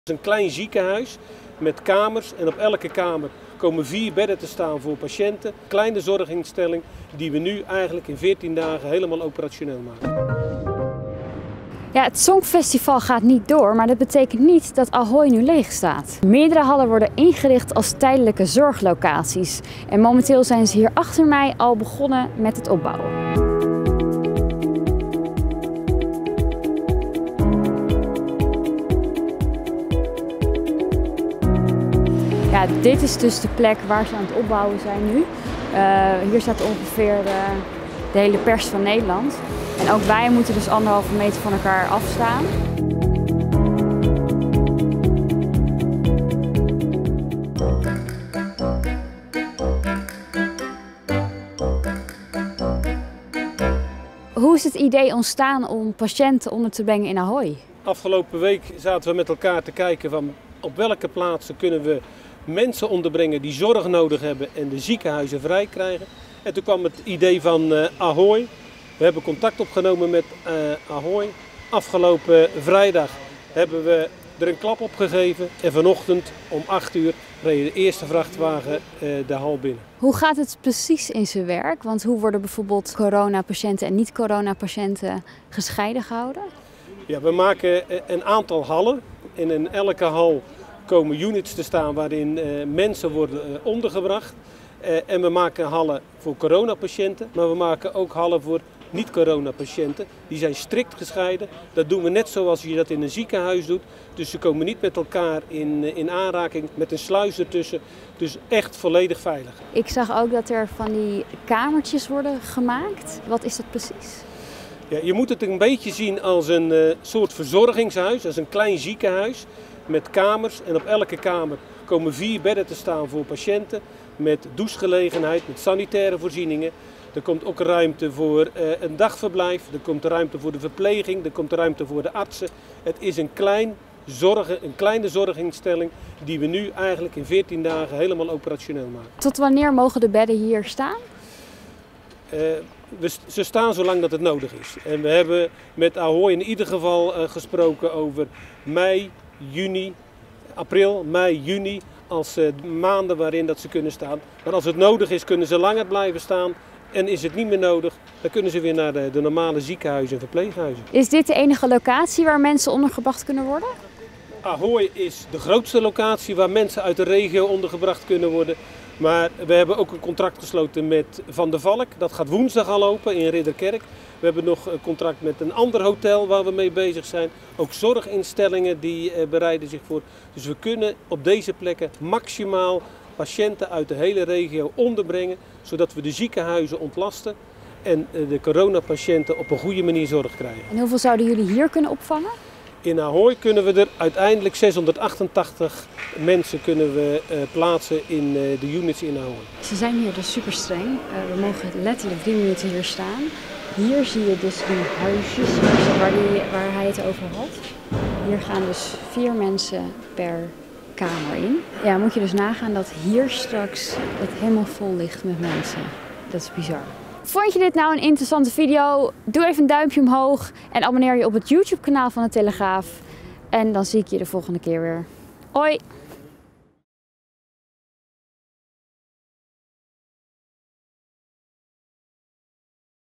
Zo wordt Ahoy omgebouwd tot ziekenhuis
Meerdere hallen in Rotterdam Ahoy worden deze week ingericht als tijdelijke zorglocatie voor corona-patiënten, die niet naar het ziekenhuis hoeven maar ook niet thuis kunnen blijven. Michelle neemt een kijkje.